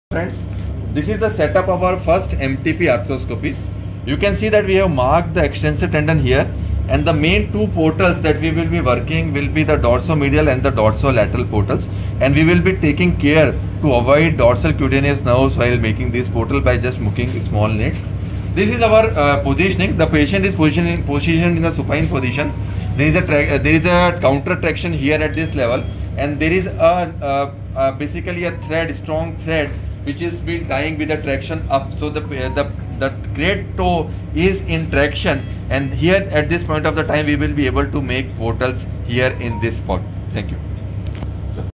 First MTP joint arthroscopy set up and Portals. Best Treatment for Great Toe pain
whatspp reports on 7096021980
Gout treatment with arthroscopy. Thumb Joint of the foot treated with Arthroscopy .

First MTP joint Synovitis can be treated well with arthroscopy .
This procedure describes the technique of First MTP arthroscopy with a ultra small scope.
advance hospitals ahmedabad is one of the few centers in india providing facility of first MTP joint or Foot thumb arthroscopy